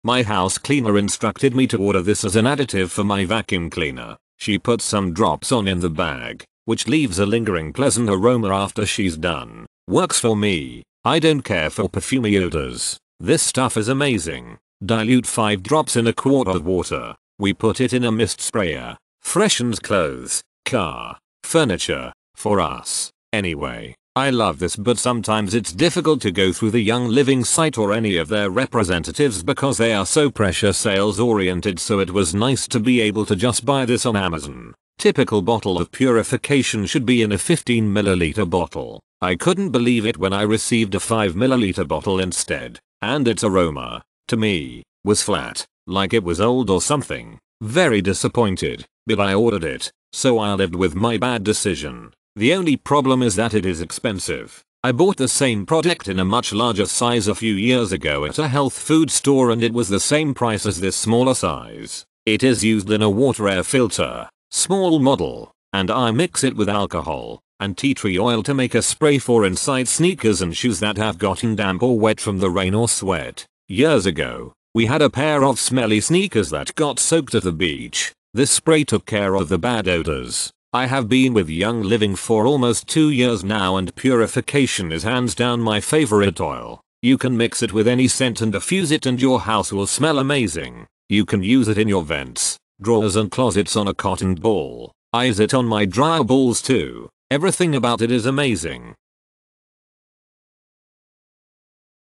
Young Living Purification Essential Oil Blend - formulated with 6 Essential Oils - review
Young Living Purification Essential Oil Blend - formulated with 6 Essential Oils - 5 ml

Main Features:
- Formulated with Six Essential Oils: The ultimate weapon to keep your home smelling fresh and clean
- Clean, Invigorating Aroma: Freshens musty and stale areas with its lively scent
- Essential Oil Blend: Combination of citronella, lavandin, lemongrass, myrtle, rosemary, and tea tree
- Aromatic Use: Infuse your entire home with bright, lemongrass-forward aromas after a cleaning routine
- Young Living: A worldwide leader in pure essential oils for your natural lifestyle